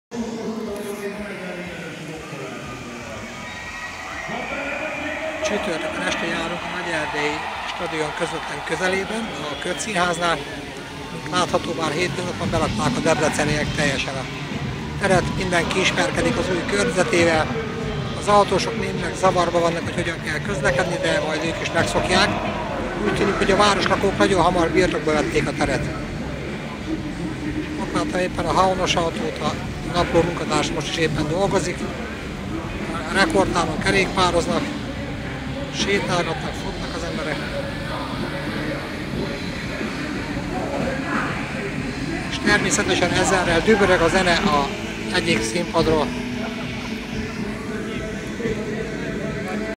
Erdőlakó debreceniek
A cívisek pillanatok alatt belaktak a parkerdő felújított részeit.